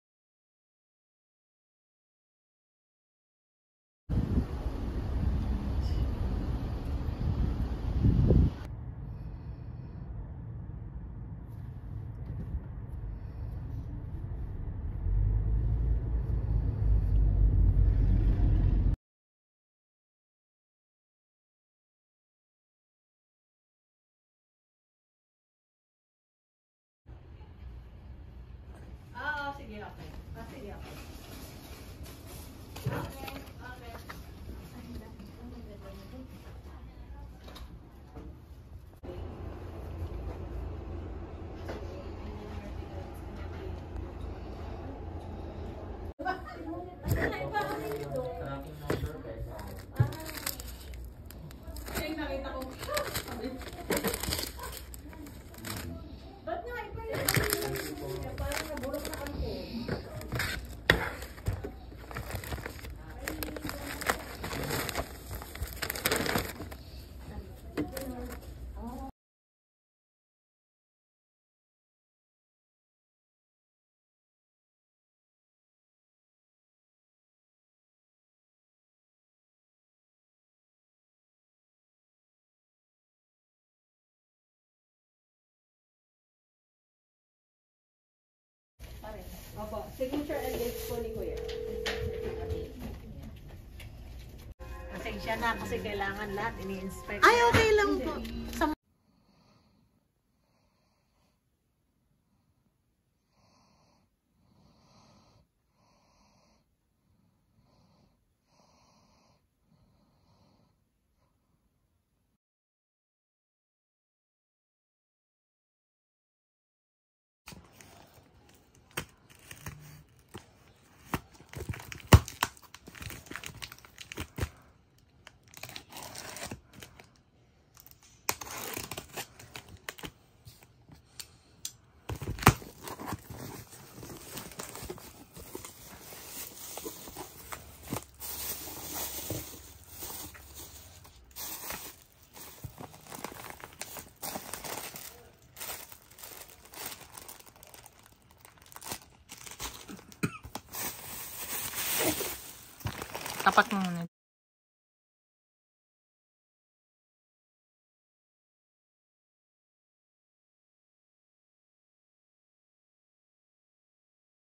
LBC Airmail package from US to Manila. Philippines for under $11 and $42 in 10 days
I like making at least 2 of these videos a year because of the changes of delivery time and price increases.
I was lucky they had a pretty good promo on the 2 times I mailed items but they have been stricter in checking each item in the box before the LBC balikbayan box is approved. This video are the 3 packages I sent so far in 2023.
So far I sent two $11 small packages as birthday gifts and 1 airbox for around $45.

I alos sent a sea cargo around the same time around April and my family recieved it on July 1st.
Im very happy with the service so far as it just made sending items to the Philippines easy for me.